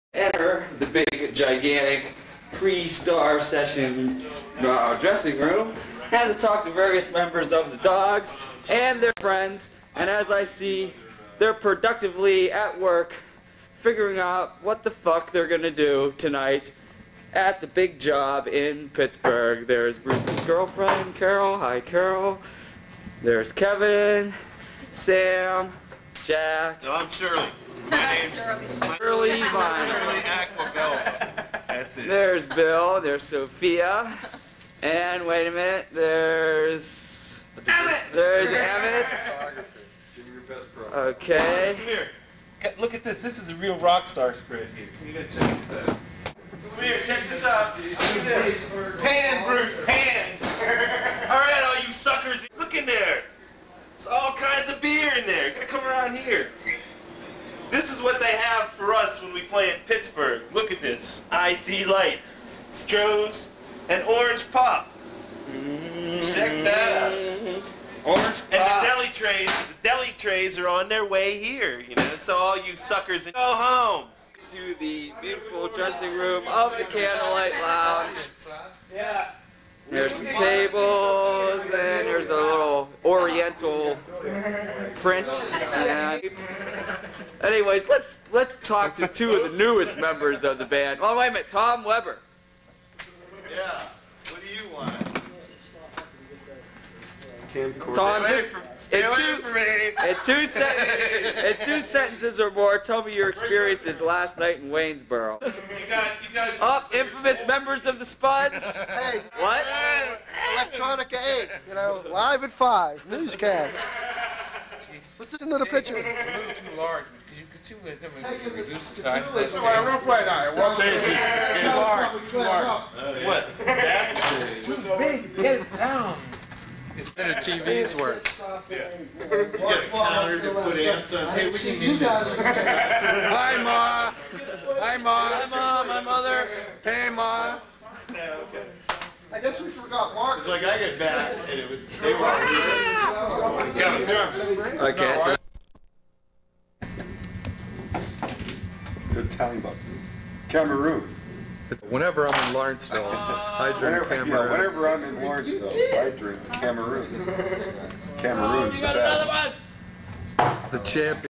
The Dogs - Out Takes - in Pittsburgh - Candlelight Lounge Lawrenceville with the Spuds
Some out takes from the Dogs video playing at the Candlelight Lounge in Lawrenceville with the Spuds